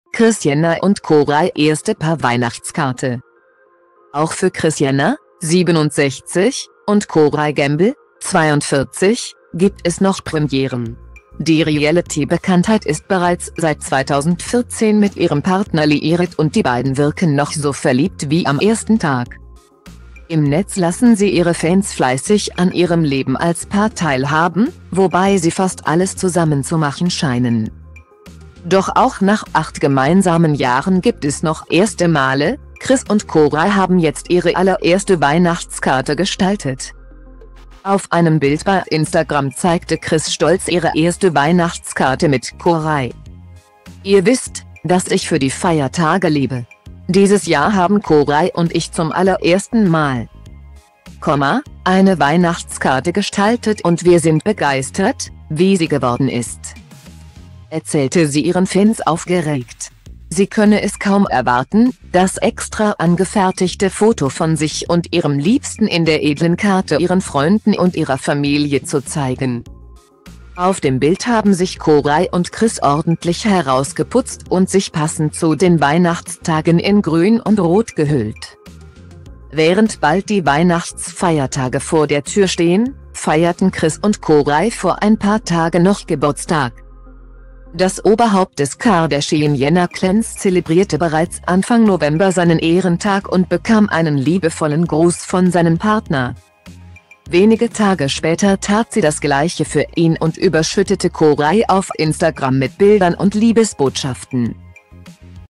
Kris Jenner und Corey erste Paar-Weihnachtskarte
Auch für Kris Jenner (67) und Corey Gamble (42) gibt es noch Premieren. Die Reality-Bekanntheit ist bereits seit 2014 mit ihrem Partner liiert und die beiden wirken noch so verliebt wie am ersten Tag.Im Netz lassen sie ihre Fans fleißig an ihrem Leben als Paar teilhaben, wobei sie fast alles zusammen zu machen scheinen.Doch auch nach acht gemeinsamen Jahren gibt es noch erste Male: Kris und Corey haben jetzt ihre allererste Weihnachtskarte gestaltet.